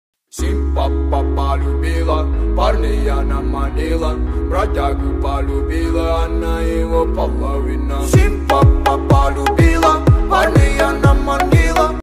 The Radiant Heroes: White Armor Chronicles ⚔️🦸‍♂️
Dive into the dazzling world of super heroes clad in gleaming white armor in this captivating short film. Follow their valiant journey as they unite to defend the universe against imminent threats. ⚡🌟